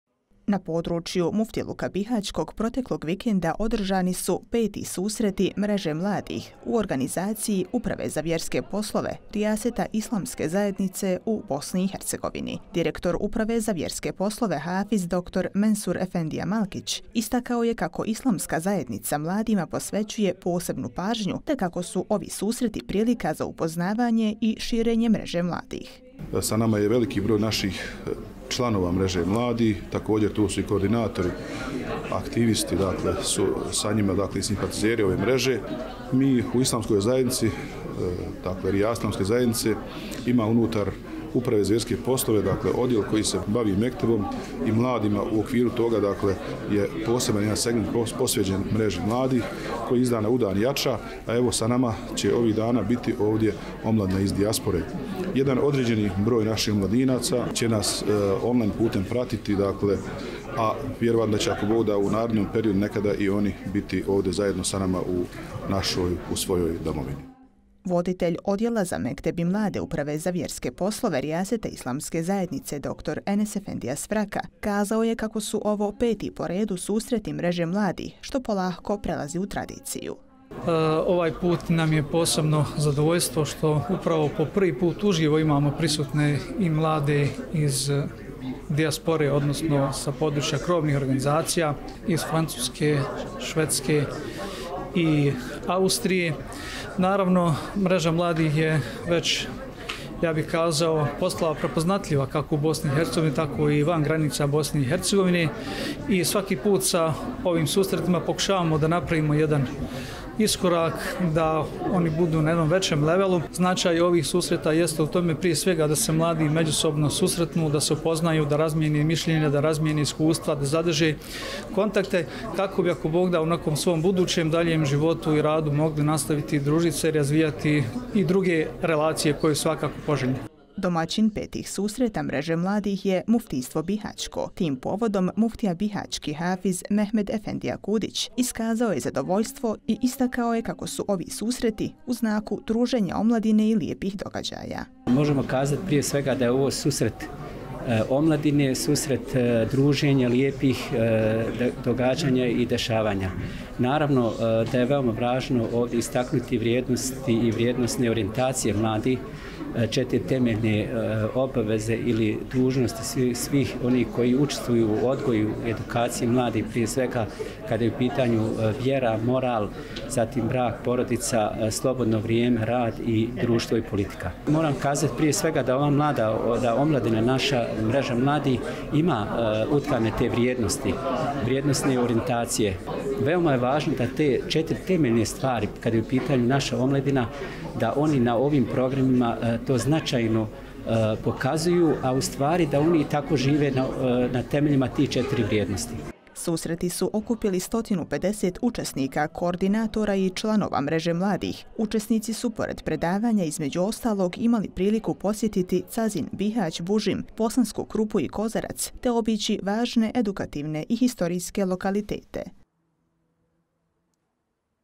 ODRŽANI PETI SUSRETI MREŽE MLADIH
Na području Muftiluka bihaćkog proteklog vikenda održani su peti susreti Mreže mladih u organizaciji Uprave za vjerske poslove Rijaseta Islamske zajednice u Bosni i Hercegovini.
Tema susreta je: "Mladi - baštinici kulturnog i nacionalnog naslijeđa".
Susreti su okupili 150 učesnika, koordinatora i članova Mreže mladih. Učesnici su pored predavanja, između ostalog imali priliku posjetiti Cazin, Bihać, Bužim, Bosansku Krupu i Kozarac, te obići važne edukativne i historijske lokalitete ovog dijela domovine.
Prilog pripremila: Amina Svraka

 - REUPLOAD ZABRANJEN –
Pratite: